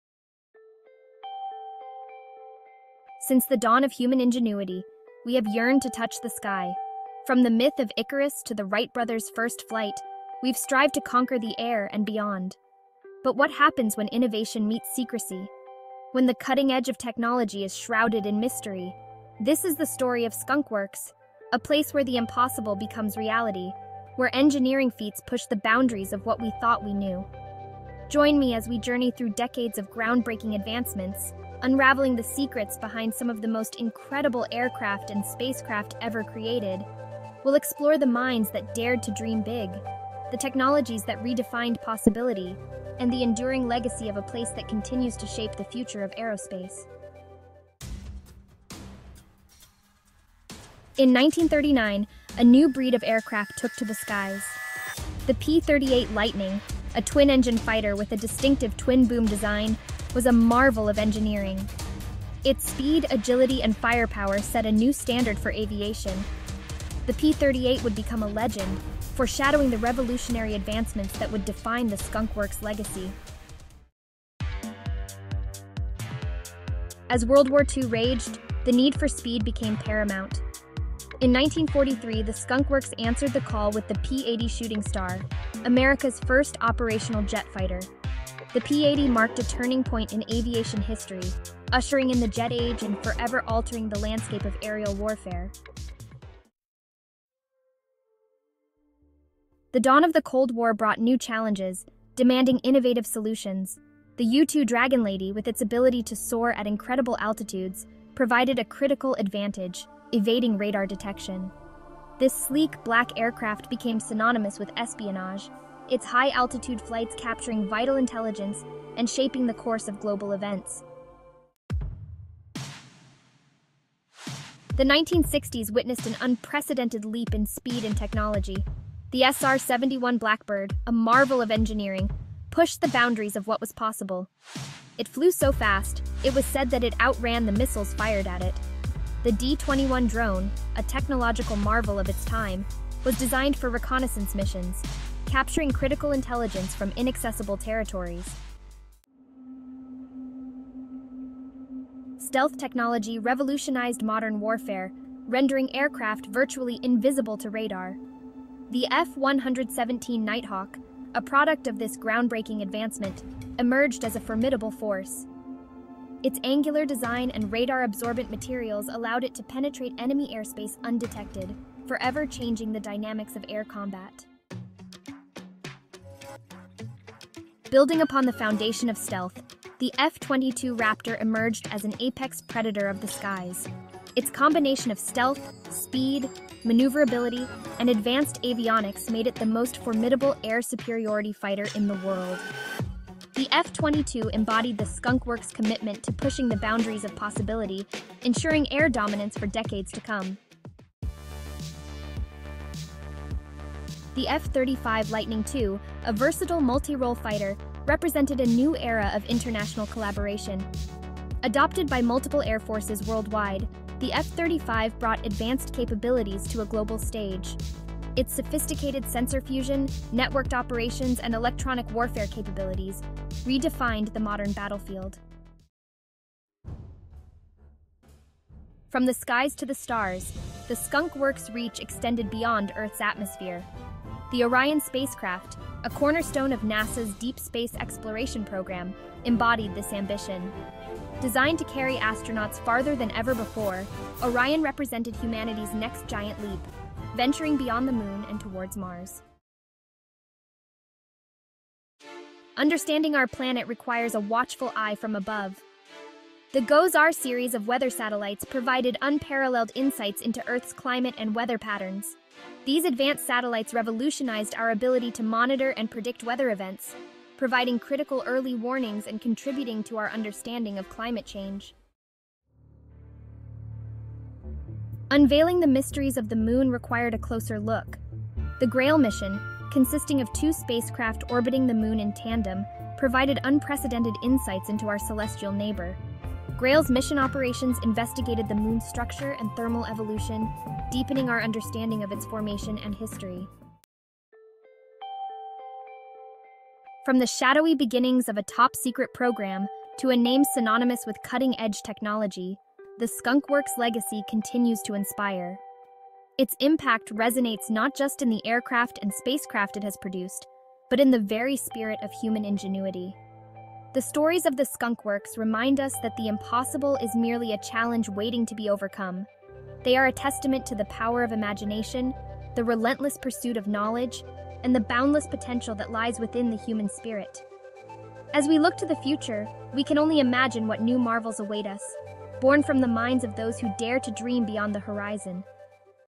Top10 Secret Tech: Lockheed Martin's Skunk Works Legacy
Top Secret Tech: The Skunk Works Legacy by Just in Time Top 10s
TIMELINE:
00:00:00 - The Skunk Works Legacy
00:00:47 - P-38 Lightning
00:01:13 - P-80 Shooting Star
00:01:36 - U-2 Dragon Lady
00:02:01 - SR-71 Blackbird and D-21 Drone
00:02:28 - F-117 Nighthawk
00:02:54 - F-22 Raptor
00:03:20 - F-35 Lightning II
00:03:45 - Orion Spacecraft
00:04:09 - GOES-R Series
00:04:31 - GRAIL
00:04:53 - A Legacy of innovation